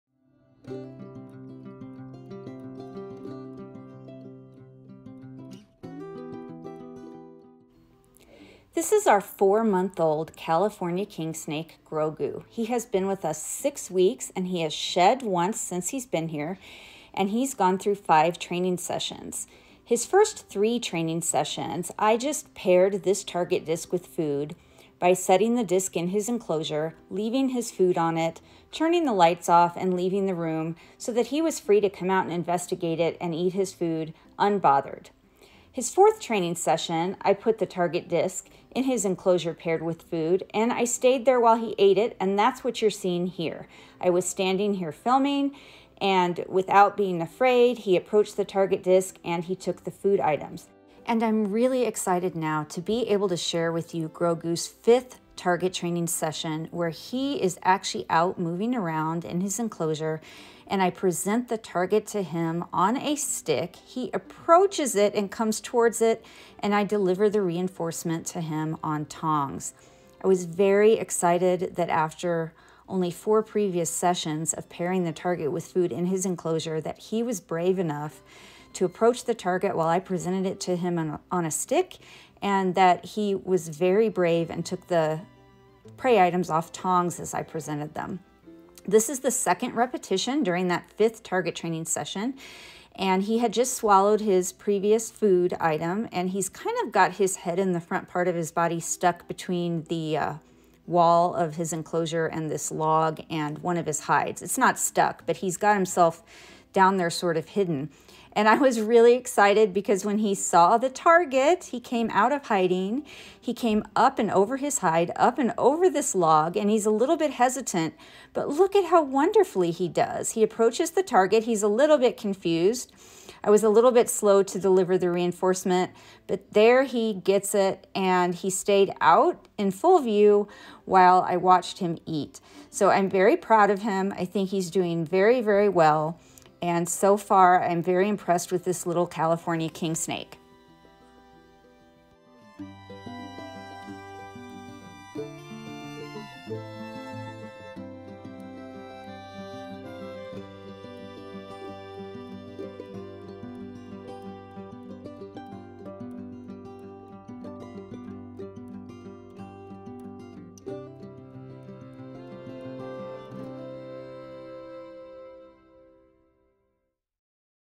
California Kingsnake Training Session Five!
Grogu is our 4-month old California Kingsnake. He was hatched on 15 September 2020 and has been here 5 weeks, 4 days. He was produced by VMSherp right here in Colorado.  Grogu’s first 3 training sessions consisted of the target disc being paired with food, placed in his enclosure, and him being left alone to interact with and investigate it. He came out while we were gone and ate his food each time after which we removed the empty disc. His 4th session consisted of me pairing the target disc with food, placing it in his enclosure, and remaining nearby. Grogu came out, approached the target and ate his meal with me standing near him filming. This 5th session, I presented the target disc to Grogu on a stick while he was visible and moving around inside his enclosure, when he approached the target o delivered his reinforcement (food) to him on tongs. We did two repetitions this way and he did GREAT!